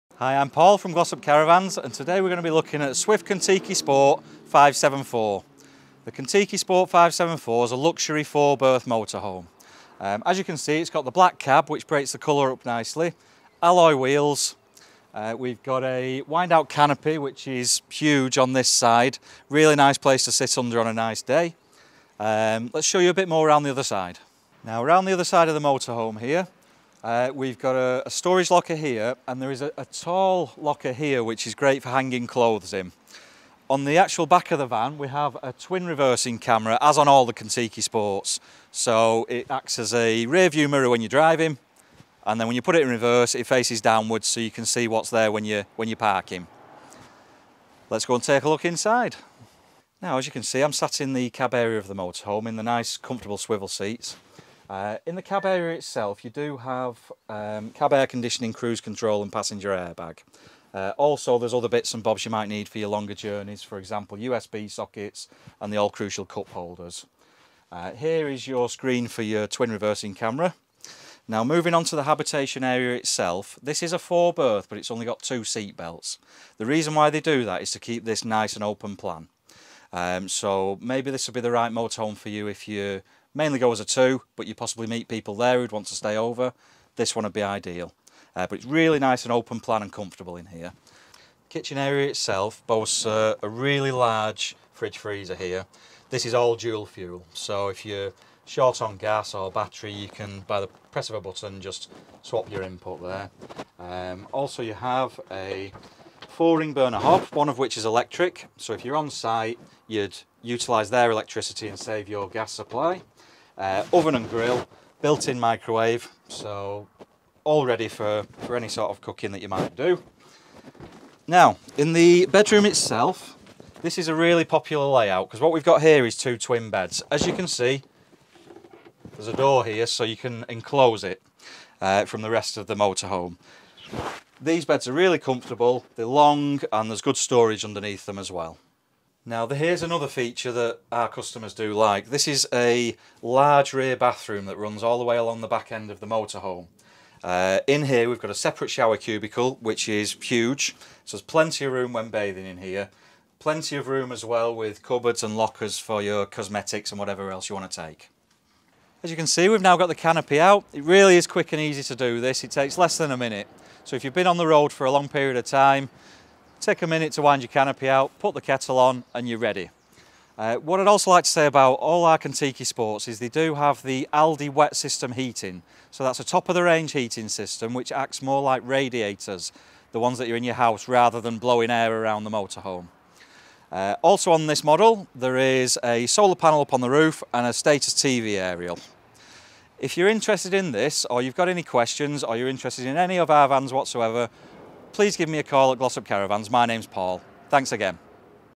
NEW Swift Kon tiki Sport 574 Lounge 2020 Motorhome Demonstration Video HD - Glossop Caravans Ltd
Additional Details : The All New Swift Kon-Tiki Sport has a layout for you with 13 different layouts to choose from, which has been designed on a Fiat Ducatto cab, which has a 2.3 litre Fiat Euro 6 Engine, with a Black Edition Fiat Chassis Cab with 16" alloy wheels and LED daytime running lights, it is also fitted with LED Marker lights and Exclusive LED rear light clusters. Built with an Aerodynamic profile with Swift's SMART construction which has a hail resistant GRP roof. When driving there are plenty of sumptuous additions to make it very easy to drive while keeping your hands on the steering wheel, including a DAB radio with Bluetooth with steering wheel controls, Cruise control, and it is fitted with a twin camera reversing camera, it is also fitted with Cab air-conditioning, and drivers and passengers airbags should the worst happen, you'll be safe. On the roof there is a large 100W solar panel to help keep your leisure battery charged, and there is a Status 570 TV aerial, design is key to this motorhome, that is why the full length awning is recessed meaning it doesn't stand proud of the Kon-tiki sport side. There is also a Pull-out Pannier Locker giving added practical storage. For all year-round usage, it has the pinnacle of heating which works just like a home central heating system, Alde Compact 3020 HE Dual fuel heating with grade 3 insulation and it gives a huge amount comfortable heating. The captain's chairs are also comfortable in the evening, with the adjustable cab reading lights. The whole motorhome has remote central locking cab and habitation door with a single key for all habitation doors and all the lighting is LED lighting throughout including the spotlights, and to help block out light there are concertina windscreen blinds and Pleated cassette blinds any flyscreens throughout. The seating area is upholstered in 'Darwin' soft furnishings which is the same as on the Swift Elegance range which is also the pinnacle of caravanning. There is a Panoramic sunroof (model specific) adding in lets in lots of extra daylight into the habitation area. The kitchen has a Thetford Thermostatic Oven and Grill, with a dual fuel hob, stainless steel microwave and the Fenix illuminated acrylic splashback, with a FENIX NTM worktop and a stainless-steel underslung kitchen sink, and a Dometic fridge/freezer. For models which have a fixed double bed there is a Duvalay Gravity Lux Memory fibre mattress, with integrated headboards. The washroom is bright and well-equipped washroom, with a separate shower cubical with a Eco-Camel showerhead. The Swift Kon-tiki sport is fitted with Swift Command which has a 7" touch screen which can control the habitation stereo, the lighting and the heating, and you can even set timers when your away from your motorhome via a smart phone.